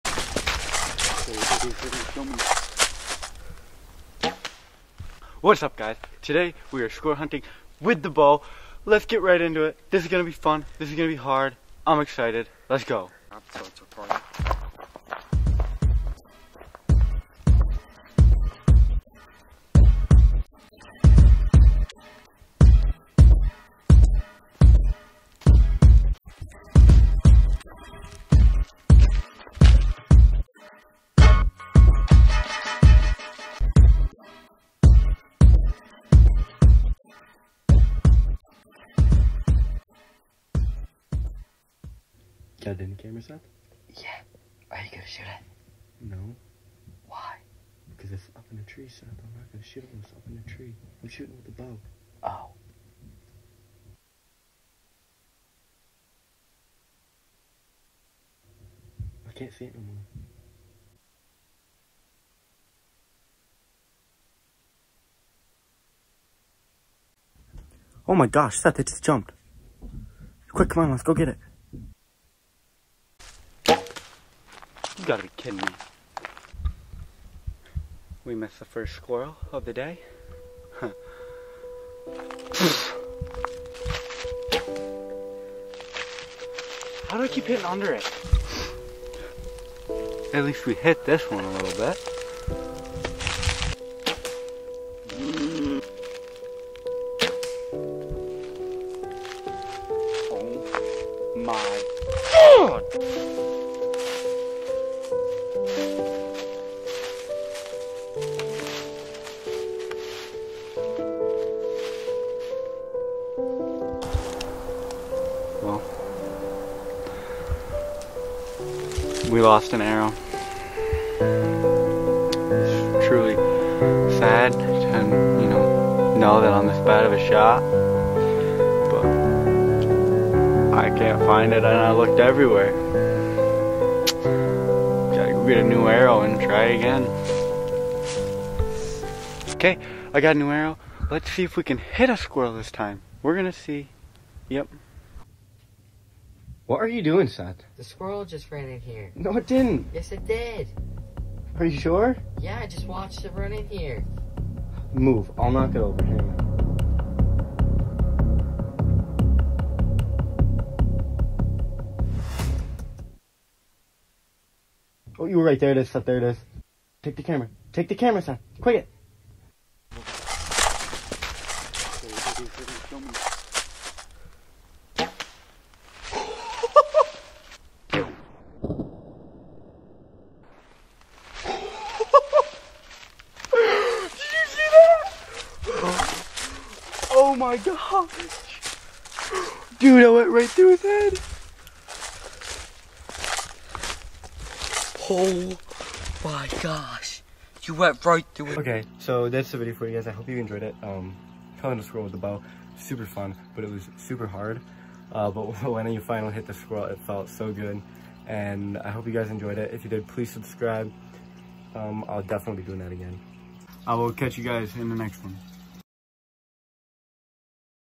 Squirrel Hunting with THE BOW!!!
In this video we squirrel hunt with the bow. If you guys like it I will do more videos like this, and if you have any ideas of videos you’d like to see put it below in the comments.